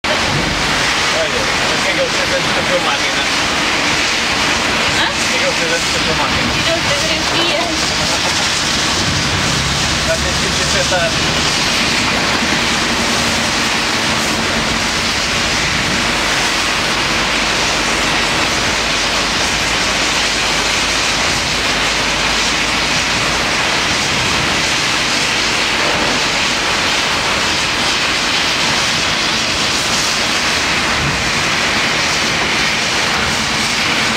Driving in tropical downpour
Watching a truck during a monsoonal torrent